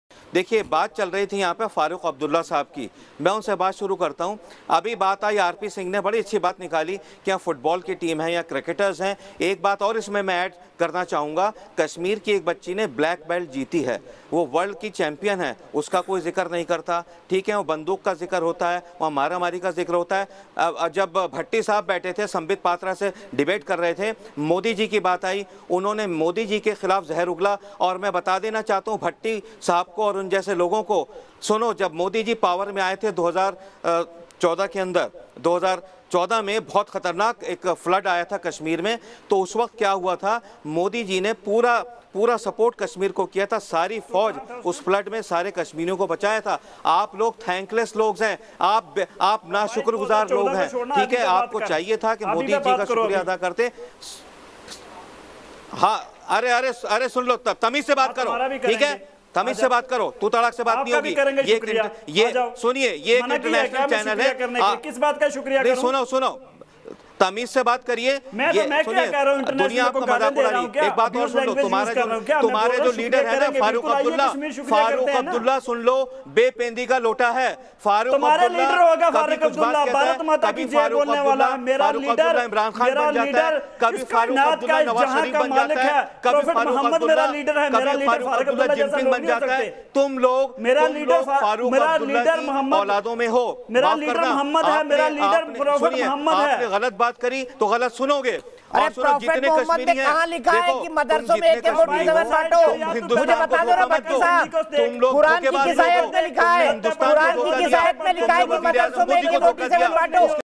बिन पेंदी का लोटा है फारूक अब्दुल्ला, मौका देख बदलता है पाला: फिरोज अहमद बख्त
मौलाना आजाद नेशनल ऊर्दू यूनिवर्सिटी के चांसलर फिरोज अहमद बख्त ने कहा कि फारूक अब्दुल्ला बिन पेंदी का लोटा है,मौका देखकर पाला बदलता है. जब पीएम मोदी 2014 में पावर में आए, उस वक्त कश्मीर में सैलाब आया था. तब मोदी जी ने पूरी फोर्स राहत और बचाव में लगा दी थी. तुम लोगों इसके लिए शुक्रगुजार होना चाहिए.

News State Apps डाउनलोड करें:
Download Android and iOS Apps for more updates: